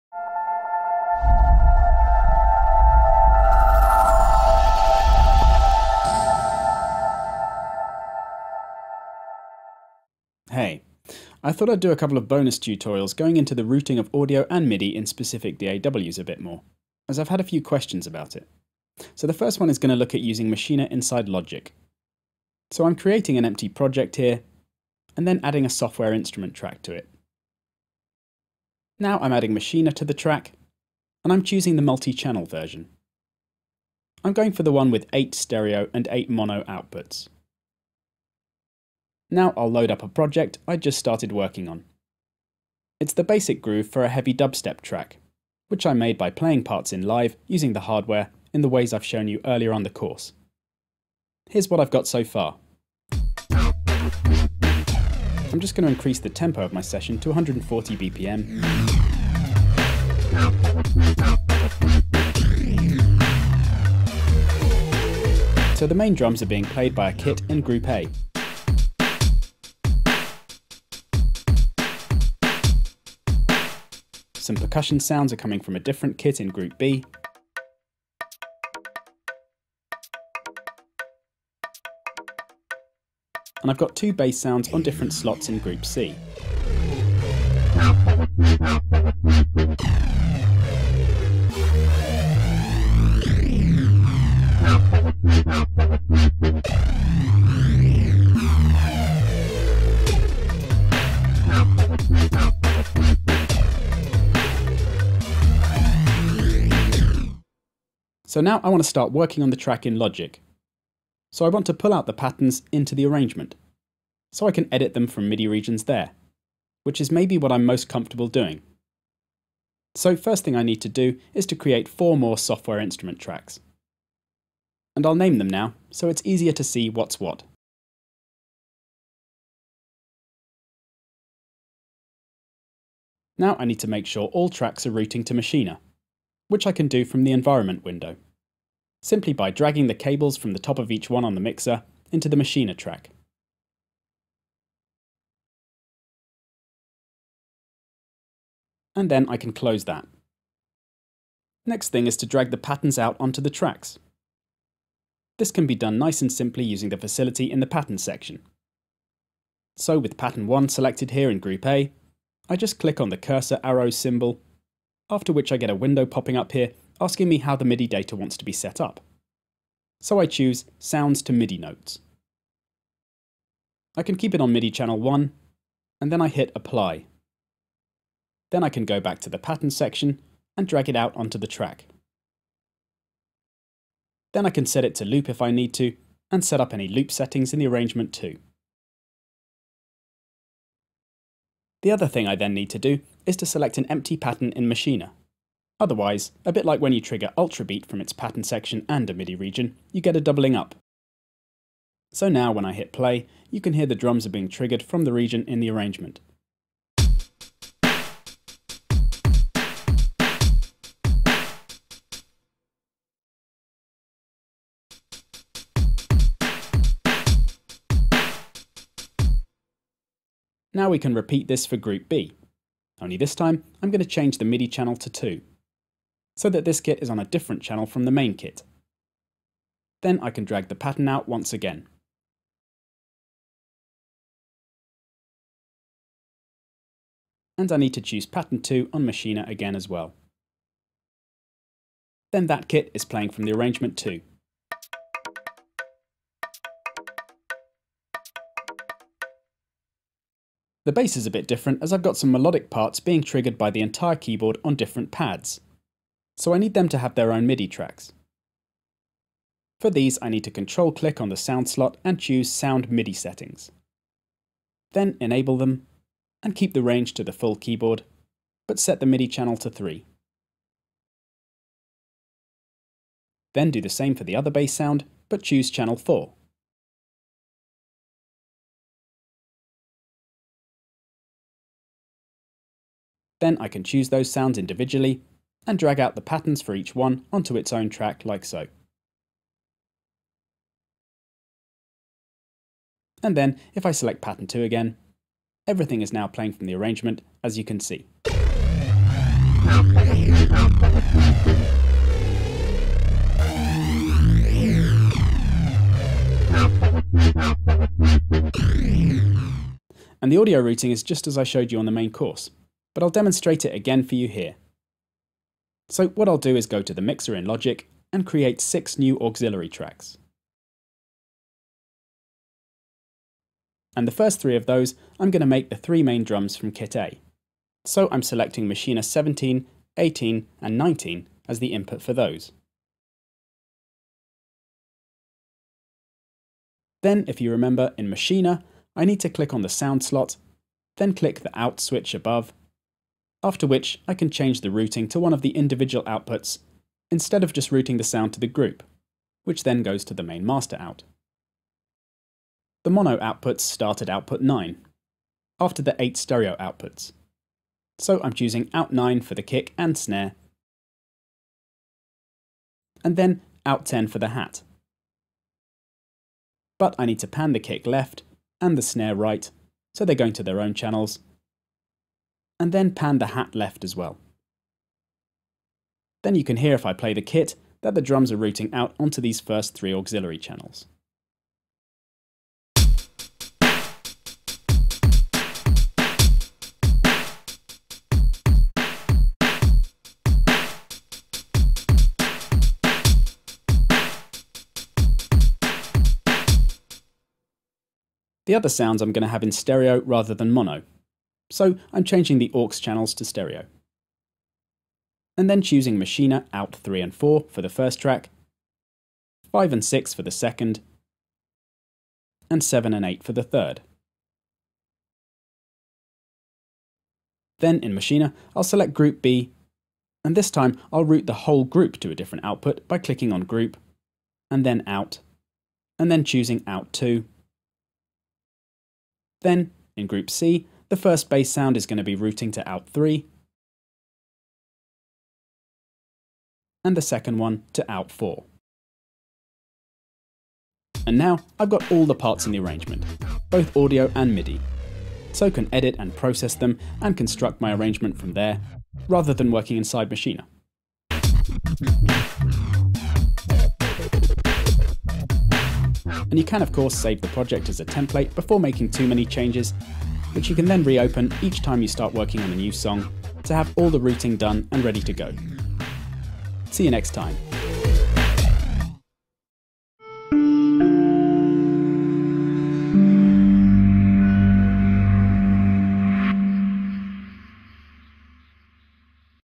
How to use Maschine in Logic Pro
Find out how to use Maschine in Logic Pro with this free tutorial taken from the Complete Maschine Guide, available

This module shows how to set up Maschine for use in Logic Pro, outlining how groups can be controlled from MIDI regions and how to route sounds from Maschine onto tracks on the mixer.